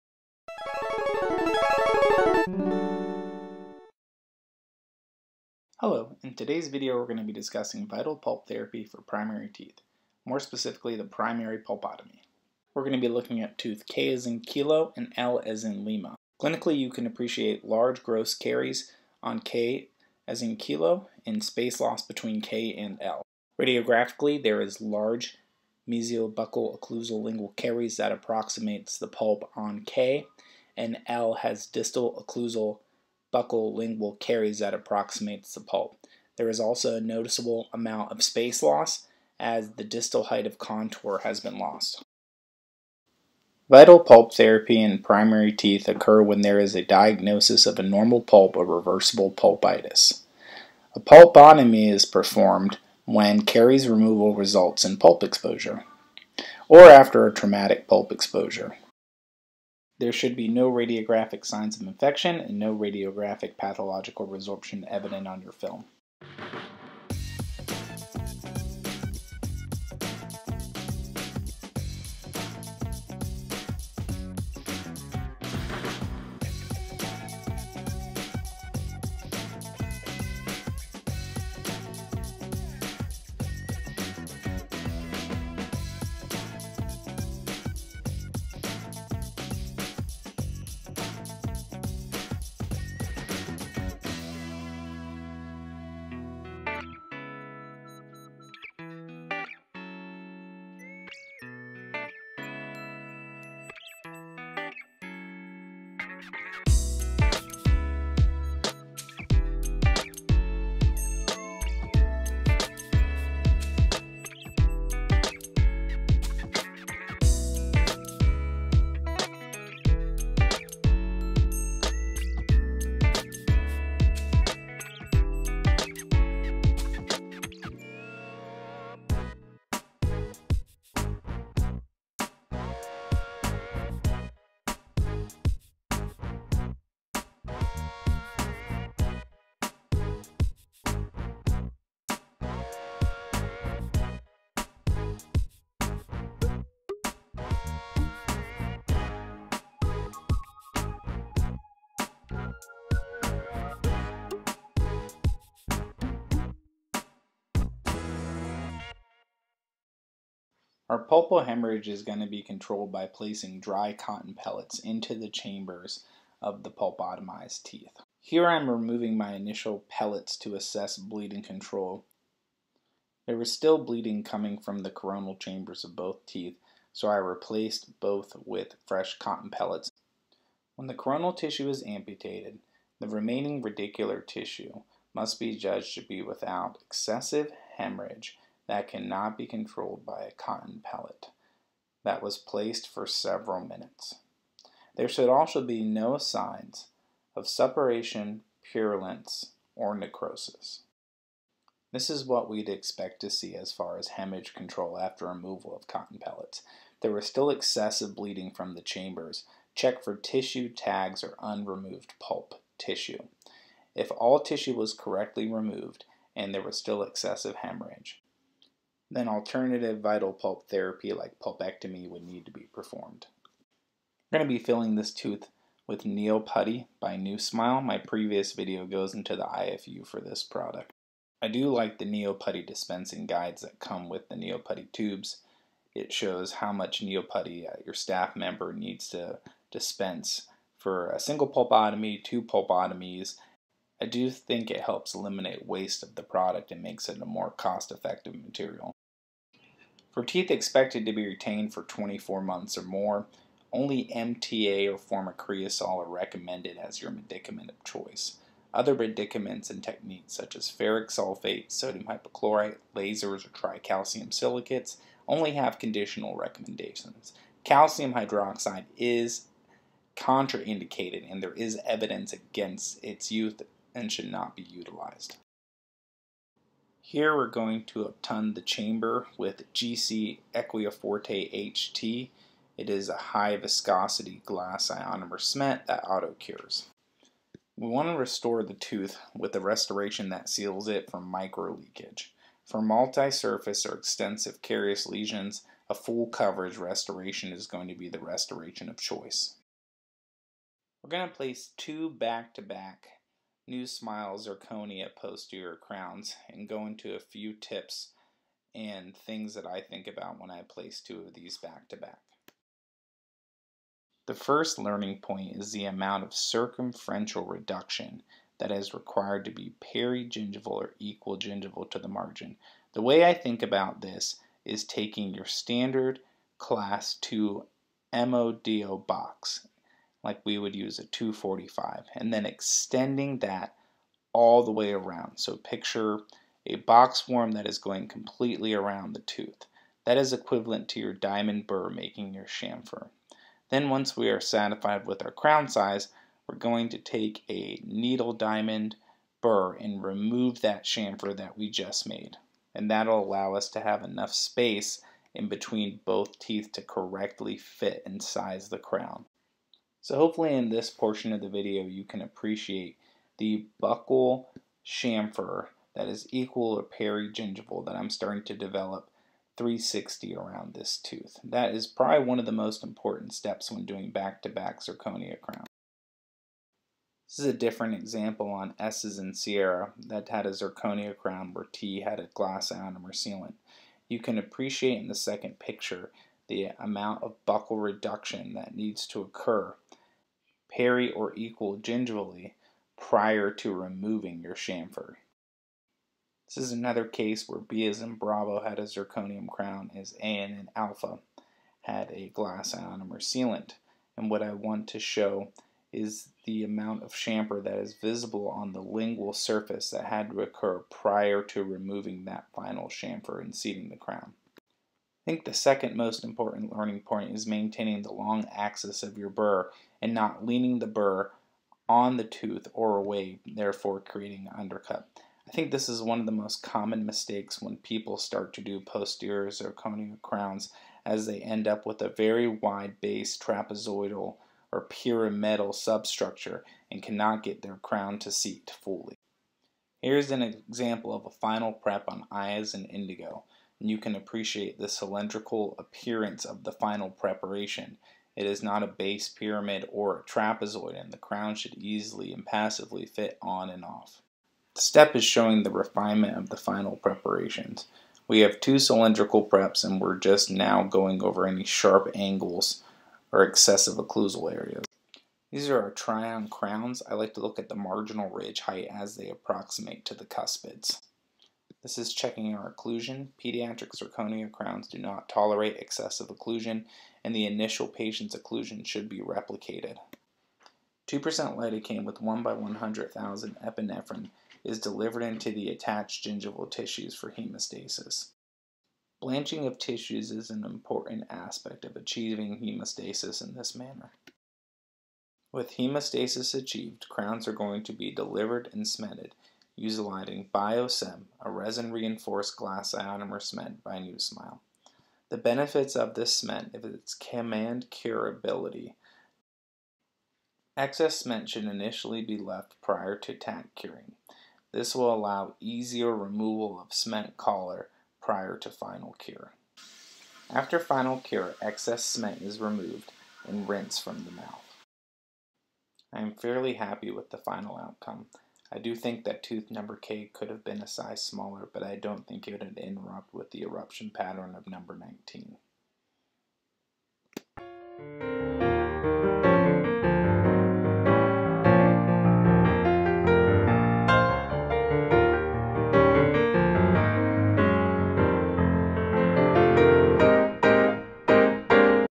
NuSmile NeoPutty Primary Pulpotomy and NuSmile Posterior Pediatric Zirconia Crown
This is a review of the AAPD guideline for vital pulp therapy for primary teeth diagnosed with a normal pulp or reversible pulpitis.  More specifically the primary pulpotomy.  This is being done with NuSmile NeoPutty, GC Equia Forte HT HVGIC, NuSmile Posterior Zirconia crowns and NuSmile BioCem.

Pulpotomy. A pulpotomy is performed in a primary tooth when caries removal results in a pulp exposure in a tooth with a normal pulp or reversible pulpitis or after a traumatic pulp exposure and there is no radiographic sign of infection or pathologic resorption. The coronal pulp is amputated, pulpal hemorrhage is controlled, and the remaining vital radicular pulp tissue surface is treated with a long-term clinically-successful medicament. Only MTA and formocresol are recommended as the medicament of choice for teeth expected to be retained for 24 months or more. Other materials or techniques such as ferric sulfate, lasers, sodium hypochlorite, and tricalcium silicate have conditional recommendations.  The AAPD’s Use of Vital Pulp Therapies in Primary Teeth with Deep Caries Lesions recommended against the use of calcium hydroxide for pulpotomy. After the coronal pulp chamber is filled with a suitable base, the tooth is restored with a restoration that seals the tooth from microleakage. If there is sufficient supporting enamel remaining, amalgam or composite resin can provide a functional alternative when the primary tooth has a life span of two years or less. However, for multisurface lesions, a stainless steel crown is the restoration of choice.

• Indications: The pulpotomy procedure is indicated when caries removal results in pulp exposure in a primary tooth with a normal pulp or reversible pulpitis or after a traumatic pulp exposure, and when there are no radiographic signs of infection or pathologic resorption. When the coronal tissue is amputated, the remaining radicular tissue must be judged to be vital without suppuration, purulence, necrosis, or excessive hemorrhage that cannot be controlled by a cotton pellet after several minutes.

• Objectives: The radicular pulp should remain asymptomatic without adverse clinical signs or symptoms such as sensitivity, pain, or swelling. There should be no postoperative radiographic evidence of pathologic external root resorption. Internal root resorption may be self-limiting and stable. The clinician should monitor the internal resorption, removing the affected tooth if perforation causes loss of supportive bone and/or clinical signs of infection and inflammation.There should be no harm to the succedaneous tooth.